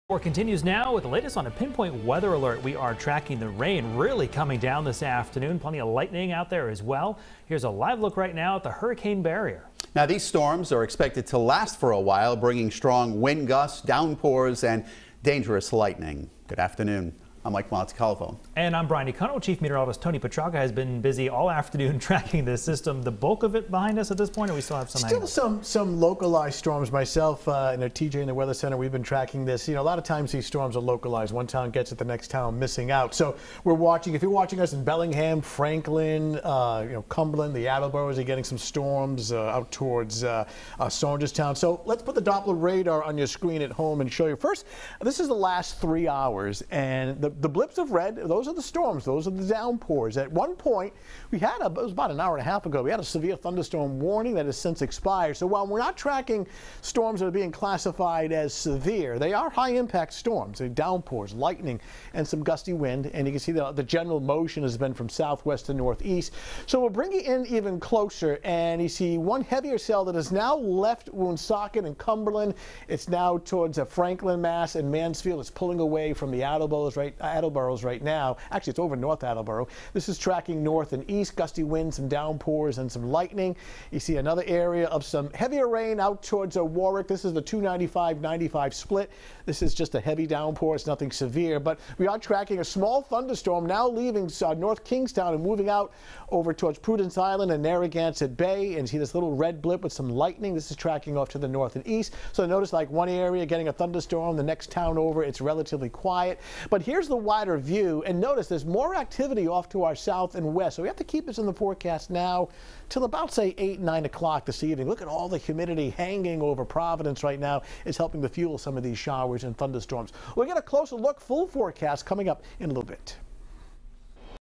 Weather Alert: Strong Showers and Thunderstorms This Evening
Weather Alert Update:  Strong Showers and Thunderstorms This Evening from 6/22/21